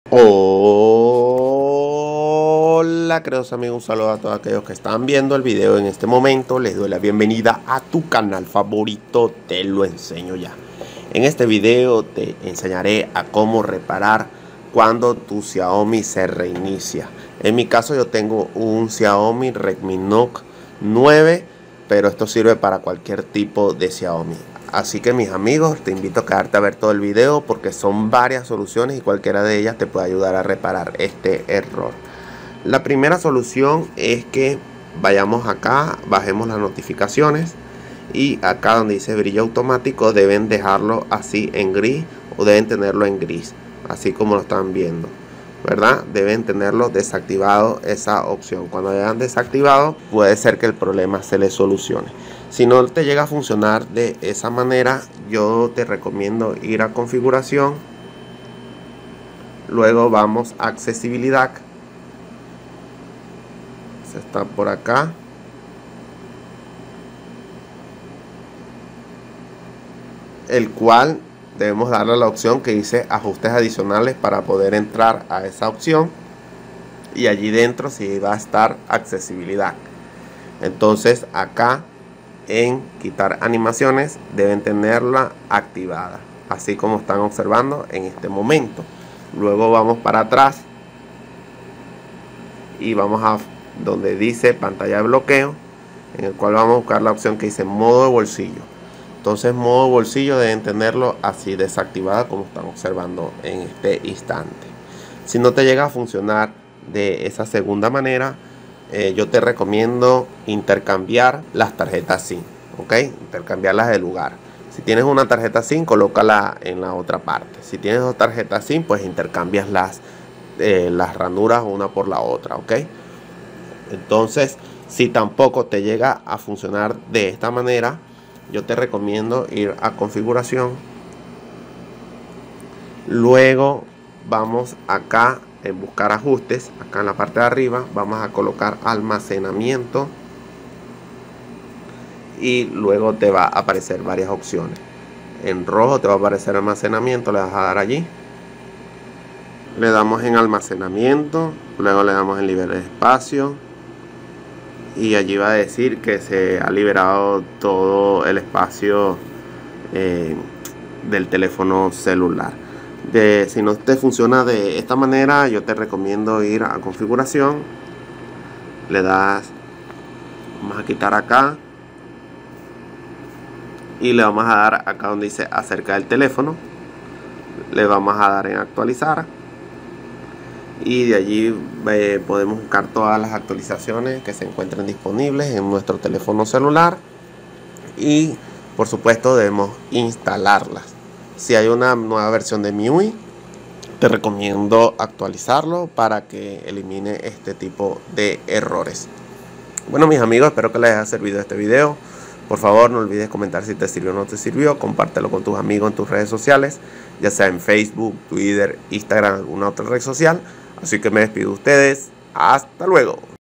Mi Xiaomi Redmi Note 8/8 Pro, Redmi Note 9/9 Pro/9S Se Reinicia Solo I 5 Soluciones 2025
En este video te enseñare a como reparar Error Mi Xiaomi Redmi Note 8/8 Pro, Redmi Note 9/9 Pro/9S Se Reinicia Solo
Videos Recomendados
💛 Como ACELERAR Celular